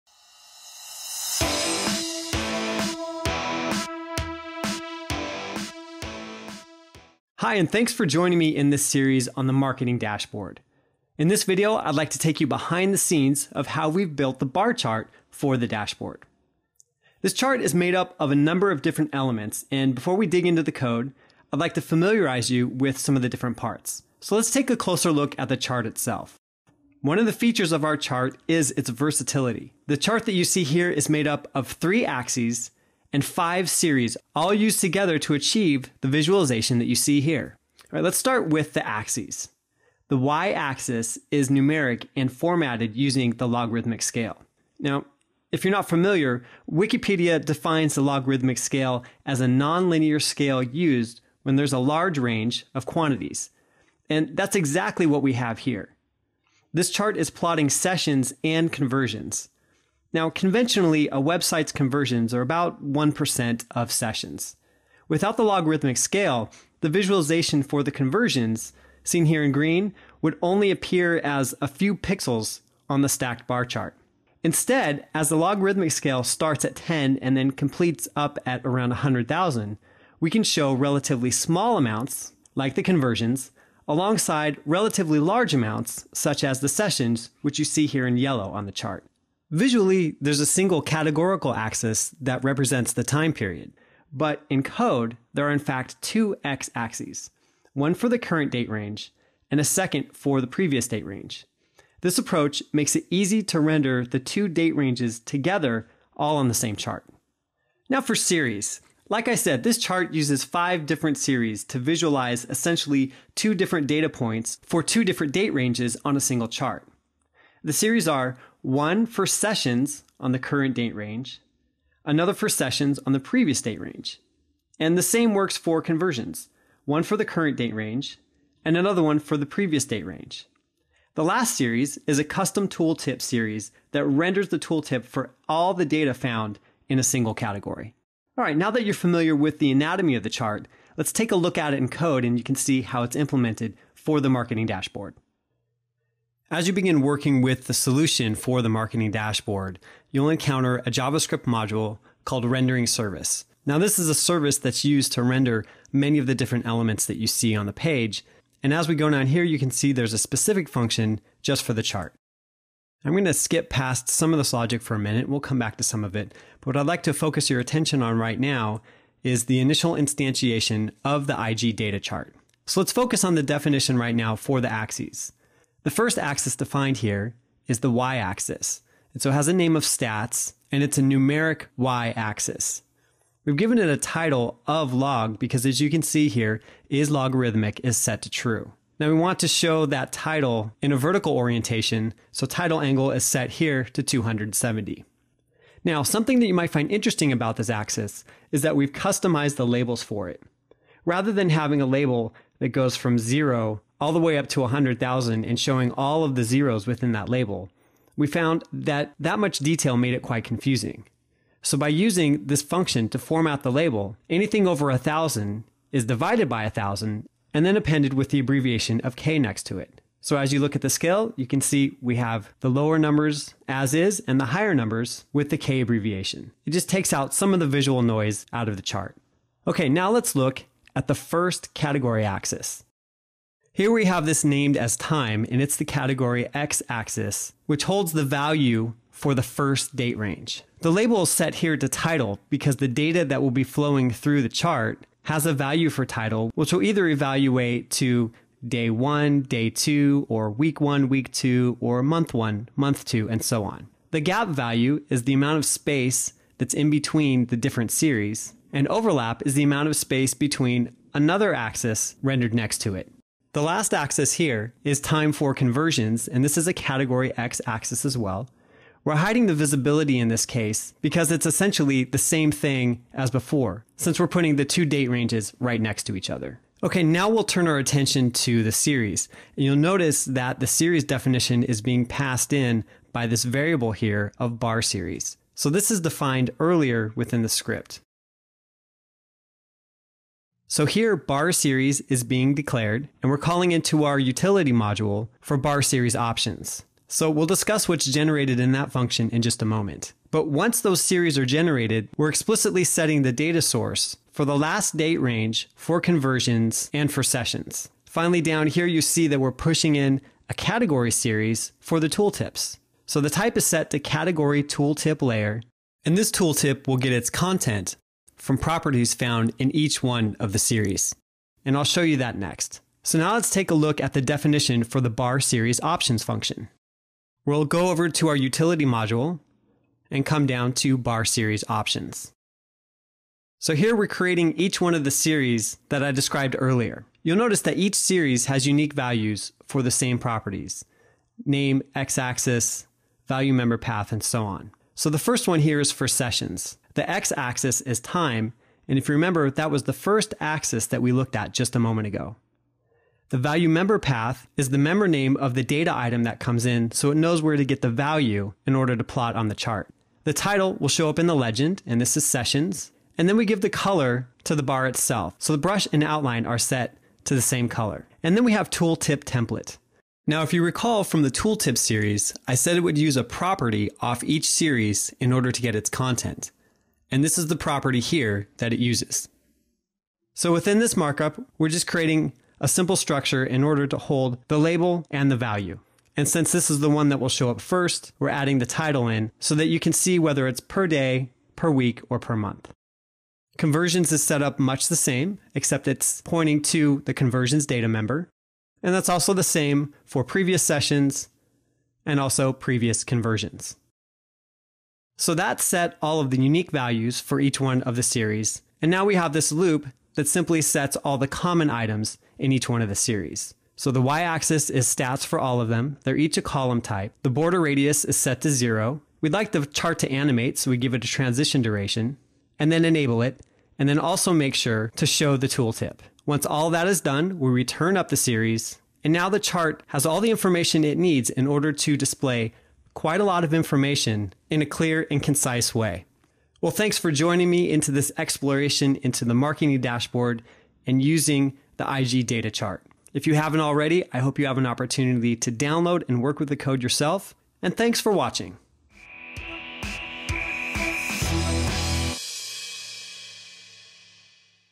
Building a jQuery Chart for a Marketing Dashboard
In this video we take you behind the scenes of who we've built the bar chart for the dashboard. The Infragistics Data chart is made up of a number of different elements, and before we dig into the code we will familiarize you with some of the different parts.  Once you're familiar with the anatomy of the chart, we'll take a look at it in code and you can see how it's implemented for the marketing dashboard.  Don't forget to download the sample and try it yourself